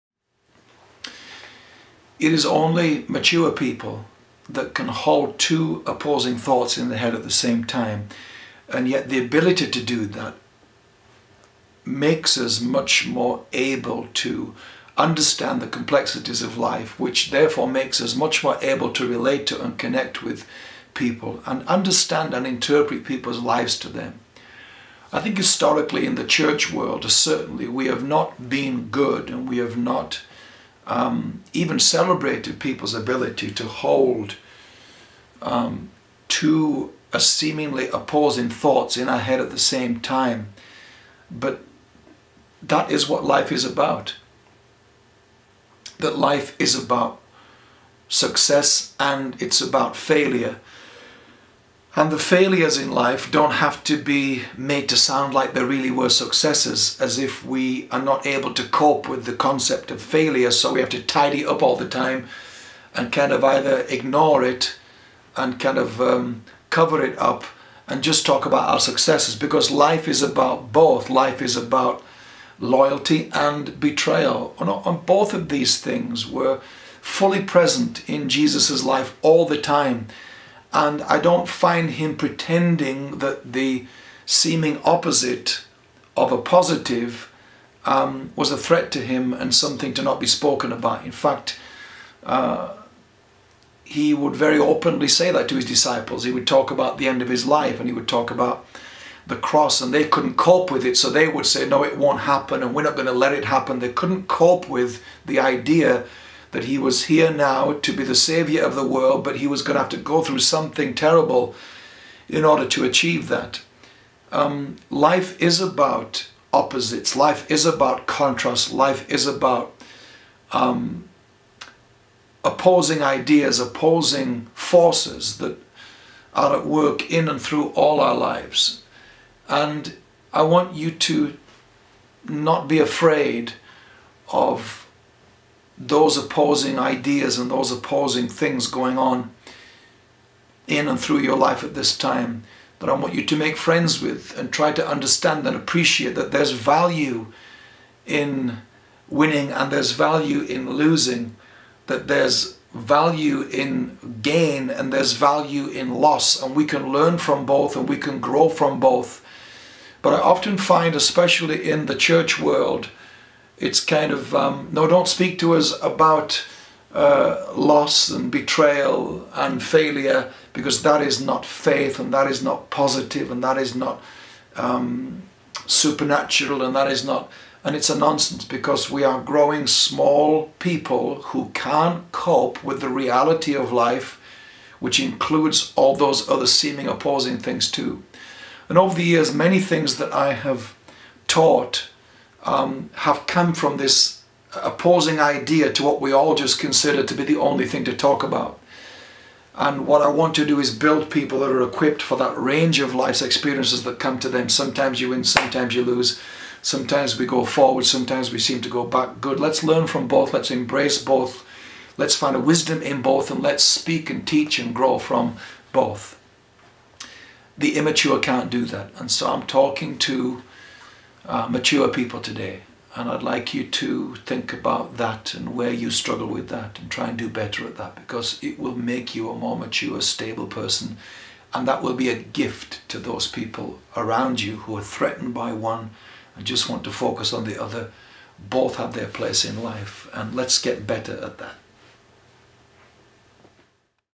Opposing thoughts
Great leaders and for that matter great humans are able to hold two opposing thoughts or ideas in their heads simultaneously.
If you can develop this skill it can become a superpower in your life.